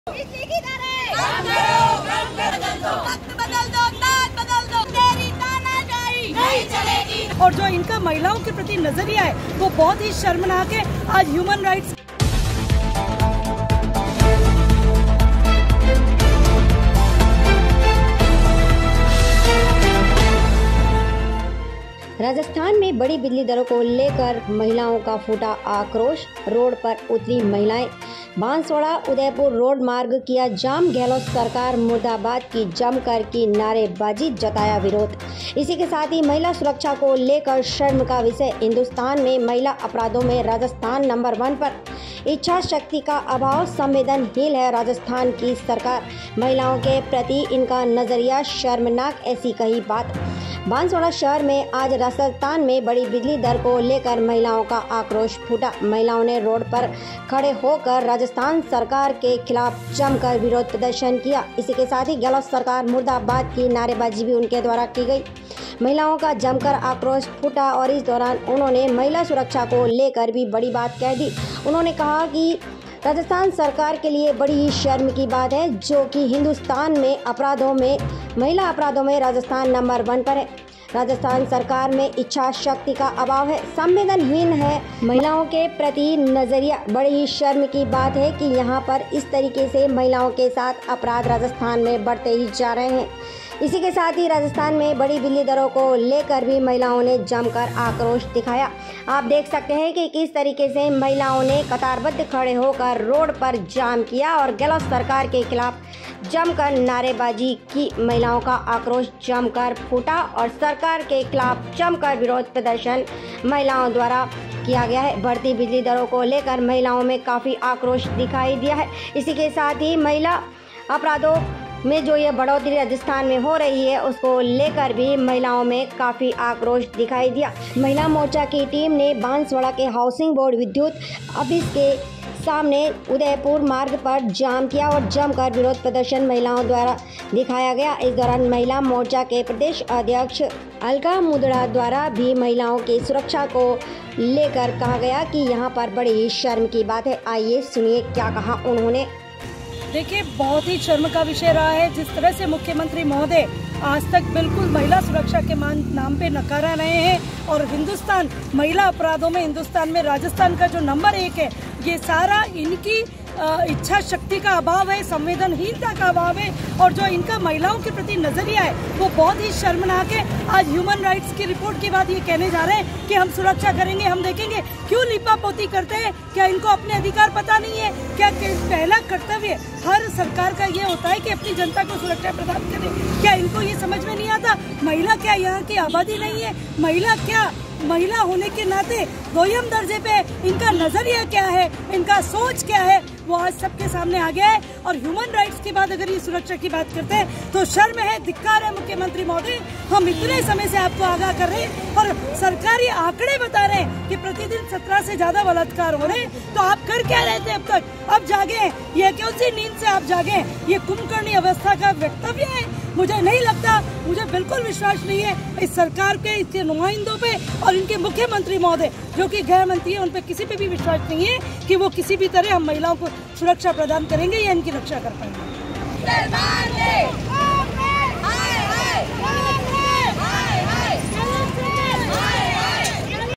सरकार के खिलाफ महिलाओं का फूटा आक्रोश रोड पर उतरी महिलाएं गहलोत सरकार मुर्दाबाद के लगाए नारे
राजस्थान में बड़ी बिजली दर को लेकर रोड पर उतरी महिला बांसवाड़ा उदयपुर रोड मार्ग किया जाम सरकार उसके लगाए मुर्दाबाद के नारे

बांसवाड़ा राजस्थान सरकार के मुखिया ने एक बार फिर से राजस्थान में बिजली की दरें बढ़ा दी है जिसको लेकर आज महिला मोर्चा की टीम ने बांसवाड़ा हाउसिंग बोर्ड विद्युत ऑफिस के सामने बांसवाड़ा उदयपुर मार्ग जाम कर जमकर प्रदर्शन किया गया इस दौरान महिलाओं ने अशोक गहलोत सरकार पर गंभीर आरोप लगाते हुए जमकर नारेबाजी की साथ ही बांसवाड़ा डूंगरपुर मार्ग को भी जाम कर प्रदर्शन किया गया इस दौरान मार्ग भी बाधित रहा महिला मोर्चा के प्रदेश अध्यक्ष अलका मूंदड़ा ने बताया कि राजस्थान में सरकार हर विषय पर विफल रही है और अब जाते जाते हैं एक बार फिर से आम लोगों के सर पर बिजली किधर बढ़ाकर वजन डाल रही है इस मामले को लेकर महिला मोर्चा की टीम रोड पर चक्का जाम कर प्रदर्शन किया और बढ़ी दरों को वापस लेने की मांग की

बाइट महिला मोर्चा प्रदेश अध्यक्ष अलका मूंदड़ा